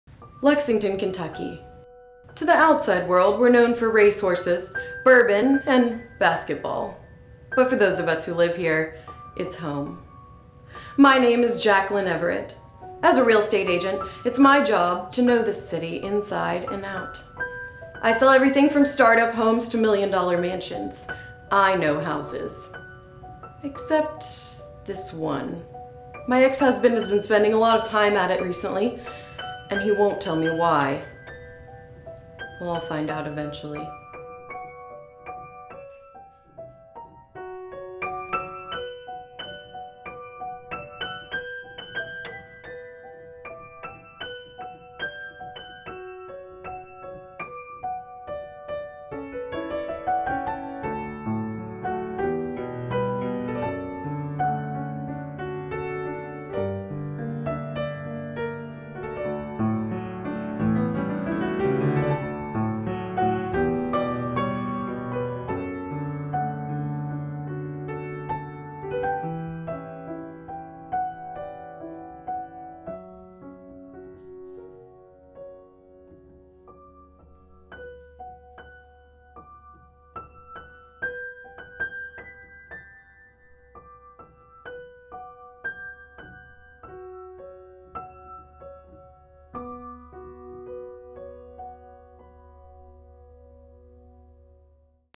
Blue Shirts - Book Trailer (Jaclyn Everett)
Sean Everett is having a tough week. After finalizing a bitter divorce, he loses his job as a stockbroker with the financial firm he has worked for most of his life. Broke and desperate, Sean finds himself fearing for his future - that is until an old high school friend of his comes back into his life, and makes him a very interesting offer. Blue Shirts is a novel written by Michael Crisp, who shot to fame in 2013 with his best selling book The Kentucky Bucket List.